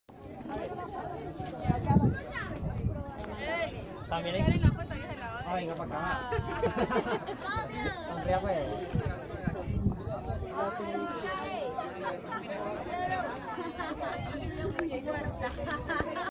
Piedra del Peñol Guatape - Colombia
Vista desde la cima de la Piedra del Peñol Guatape - Colombia